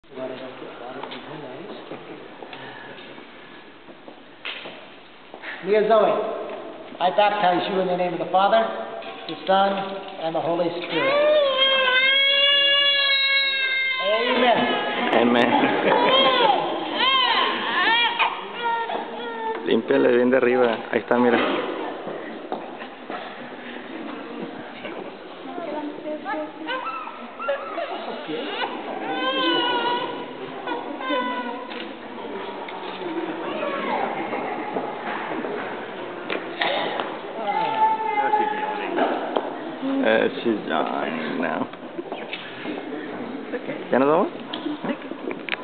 El Bautizo de Mi Bebe (Mia Zoe - 01/30/10)
aki les dejo la buena mojada ke le dio el padre al bautizar a mi bebita jajajaja!!